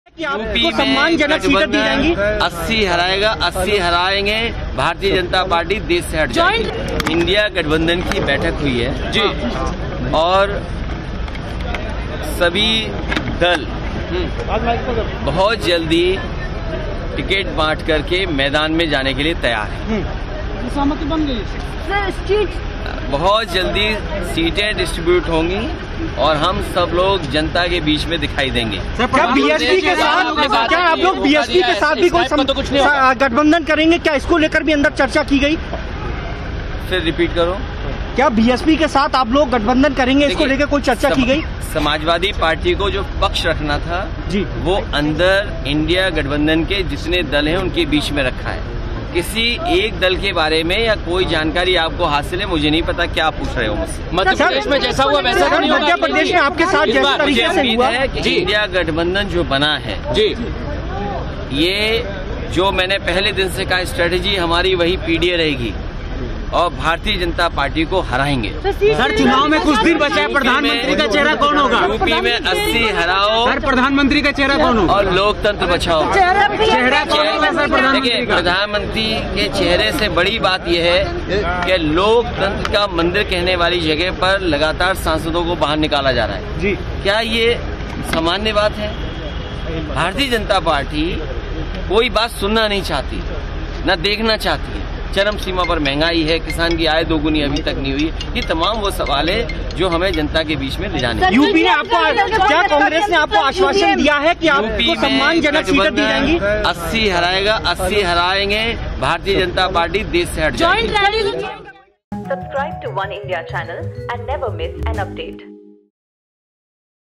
I.N.D.I.A Alliance Gears Up for Electoral Battle: Akhilesh Yadav Vows to Defeat BJP in UP | Oneindia
After the INDIA Alliance meeting, SP chief Akhilesh Yadav states, "...All parties are ready to hit the ground after the distribution of tickets very soon." He emphasises, "I have said from the first day that the strategy of INDIA Alliance will be PDA. We will defeat BJP...UP mein 80 harao aur loktantra bachao..."